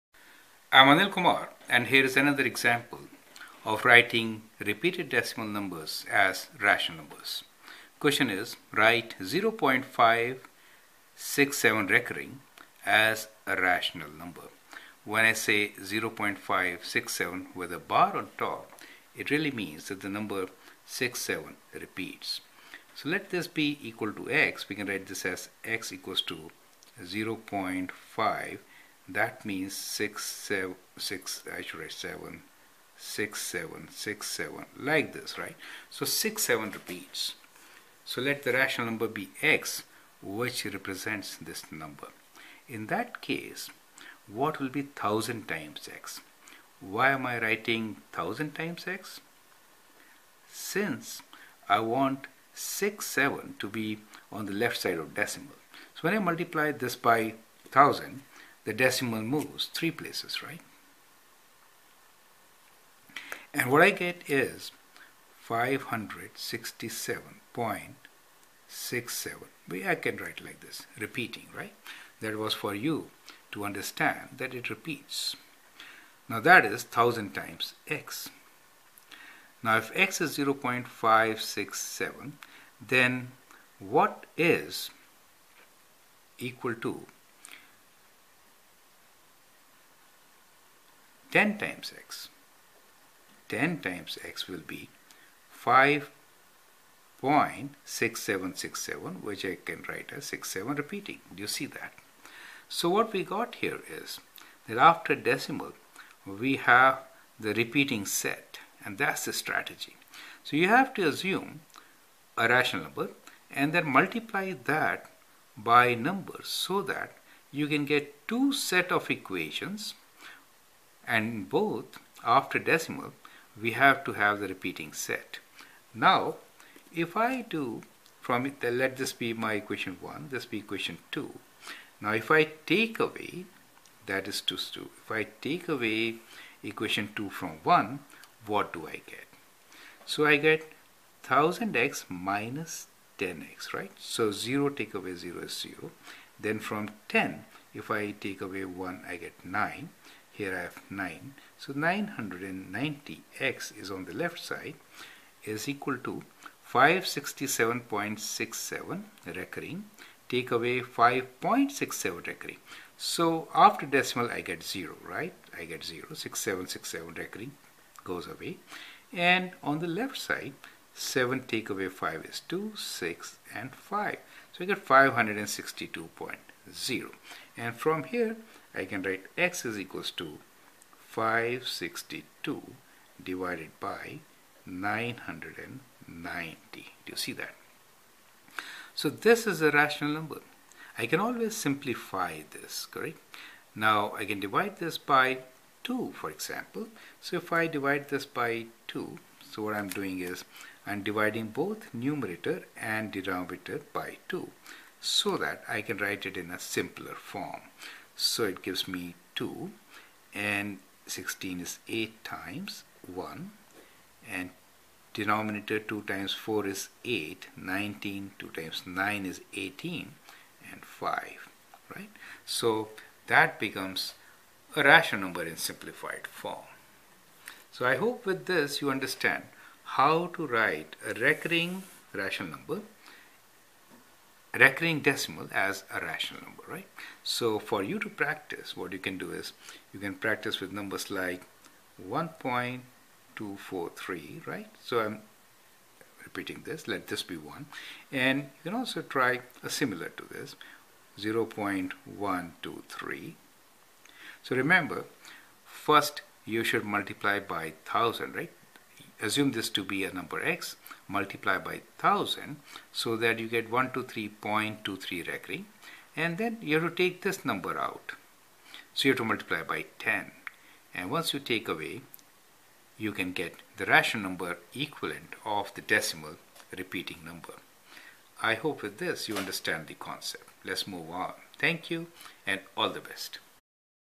Write 2 Digits Recurring Decimal 0.5676767 as Rational Number
Rational numbers can be written as repeating of terminating decimal numbers. Non-repeating non-terminating decimal numbers are irrational numbers. Square-root of 2 is an example. In this video you learn the strategy to writing a repeating decimal as a rational number.
How to write 1.232323... as a Rational Number?